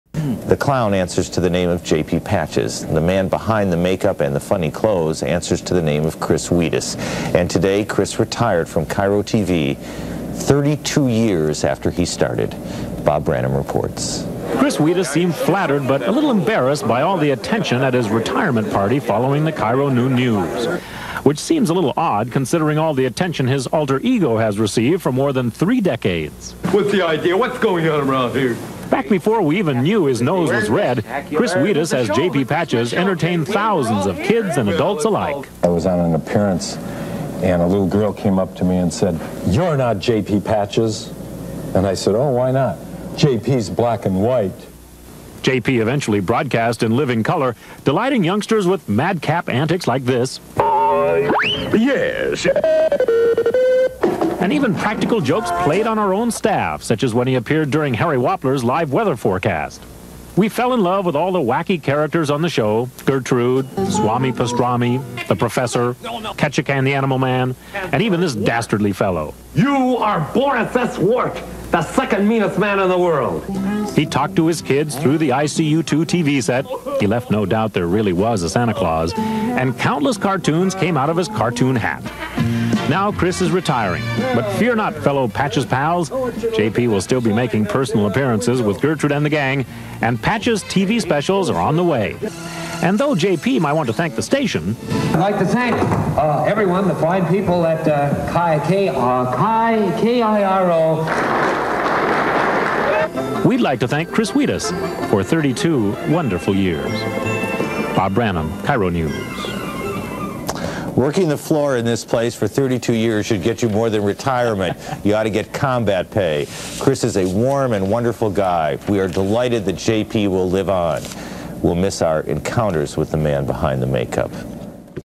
Chris Wedes, aka J.P. Patches, retires from KIRO 7 (1990)
Aaron Brown reports on the retirement of Chris Wedes, a.k.a J.P. Patches. This aired on KIRO 7 in 1990.